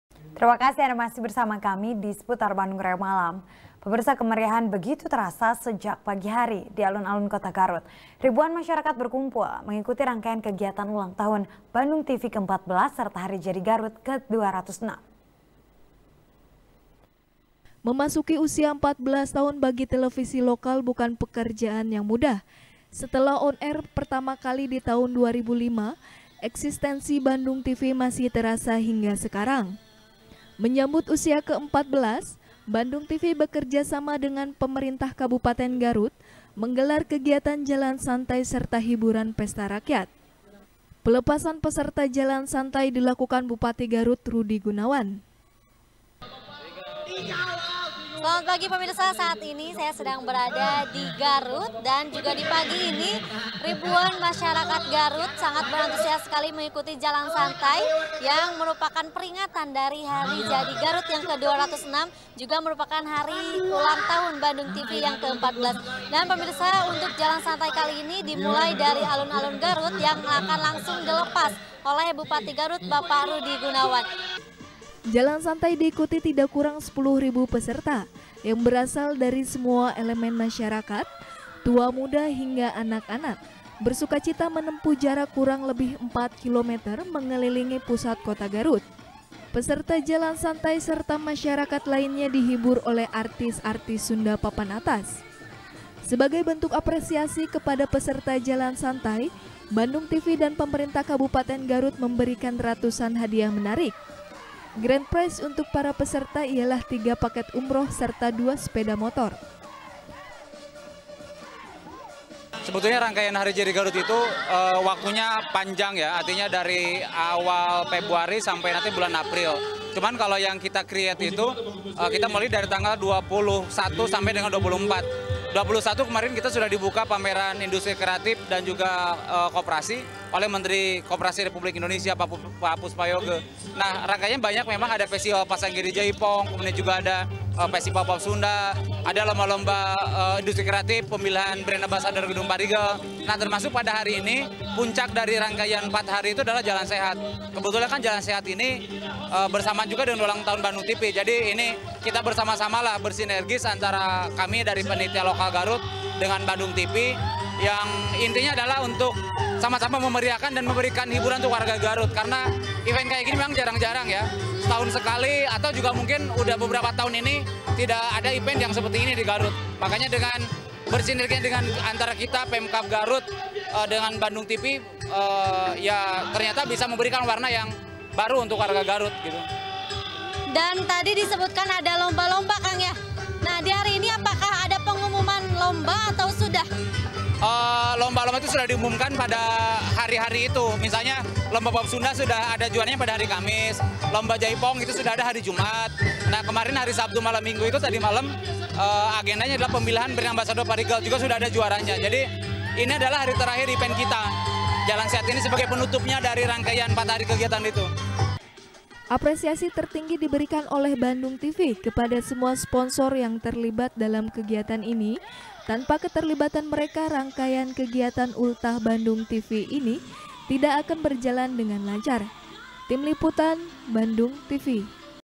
Pesta Rakyat Ultah Bandung Tv Di Garut | SBR | BANDUNGTV
SBR | BANDUNGTV | NIKEN PRATIWI | Kemeriahan begitu terasa sejak pagi hari, di alun alun kota garut. ribuan masyarakat berkumpul, mengikuti rangkaian kegiatan ulang tahun, bandung tv ke 14 serta hari jadi garut ke 206.